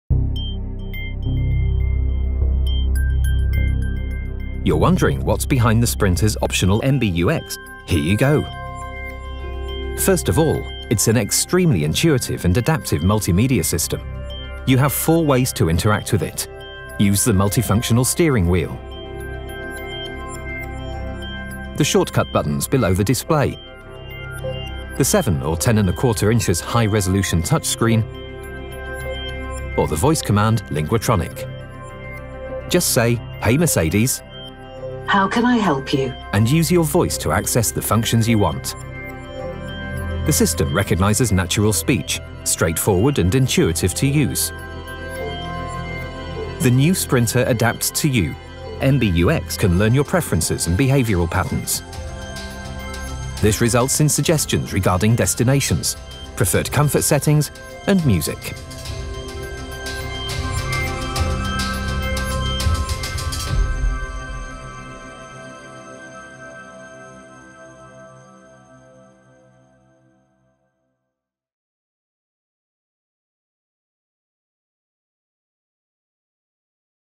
Mercedes-Benz Sprinter | This Is What MBUX Can Do
"Hey Mercedes!" – From now on you can even talk to your Sprinter, because thanks to MBUX and the intelligent voice control, the operation of the Sprinter gets even easier and more intuitive. But what exactly can MBUX do? How does the system make everyday life easier? A short portrait.